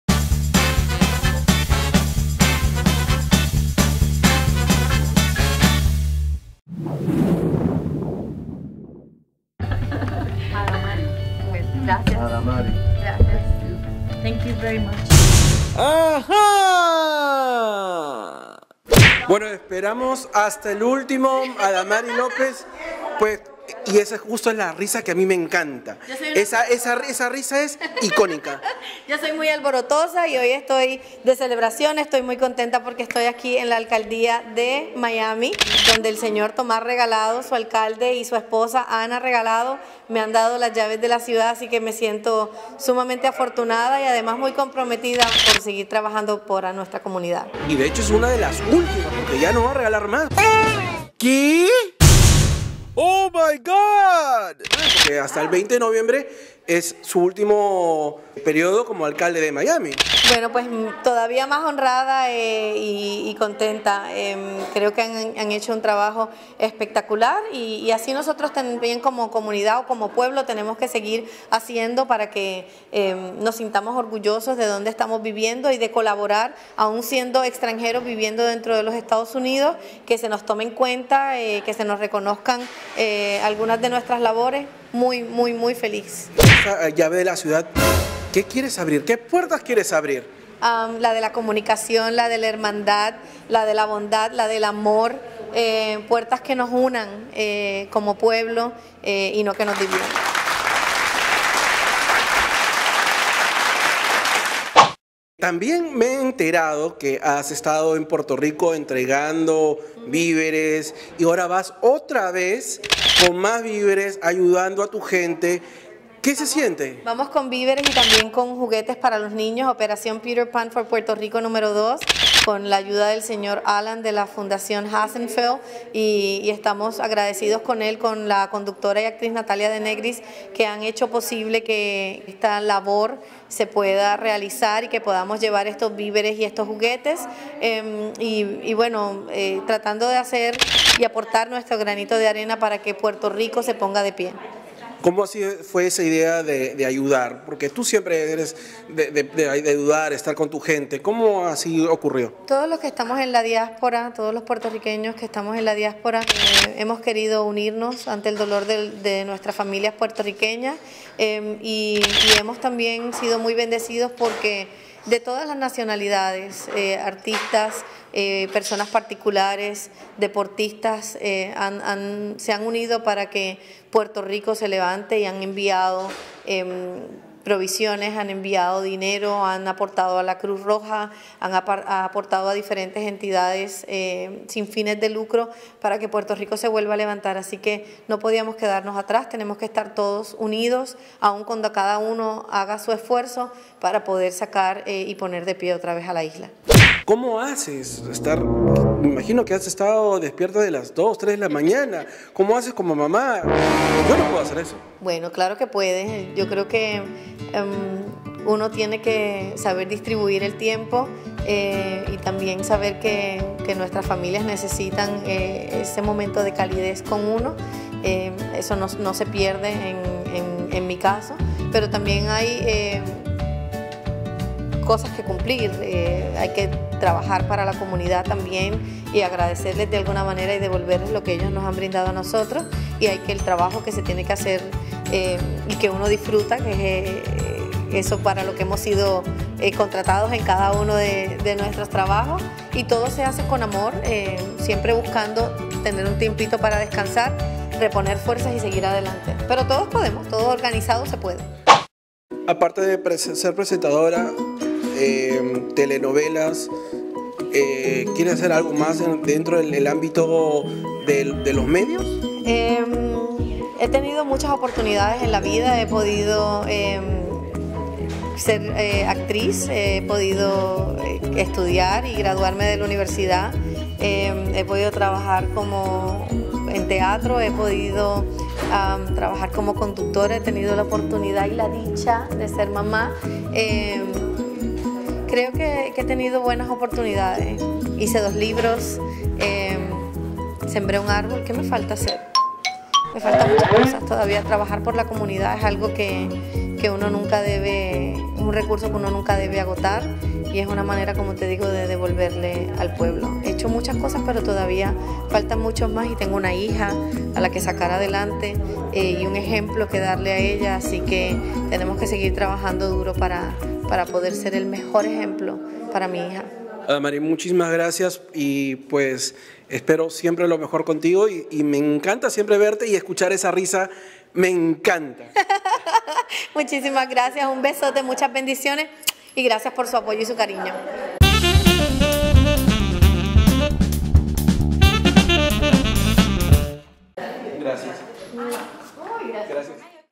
ADAMARI LÓPEZ SE ENTERÓ QUE RECIBIÓ LA ÚLTIMA LLAVE DE LA CIUDAD DE MIAMI | Tobi Te Ve
La presentadora de TV del show matutino de Telemundo Un Nuevo Día fue honrada con la entrega de las llaves de la Ciudad de Miami. Tobi Te Ve entrevistó a Adamari López y le contó que el alcalde Tomás Regalado dió la última llave de la Ciudad de Miami. Además, la también actriz boricua contó que a través de la fundación de Alan Hassenfeld y Natalia Denegri está ayudando a Puerto Rico, tras el paso del huracán María, llevando víveres a los damnificados. Asimismo, desde la Isla del Encanto colabora a que los boricuas lleguen a Miami, mientras que Puerto Rico se recupere en su totalidad.  La también actriz habló más sobre su labor humanitaria.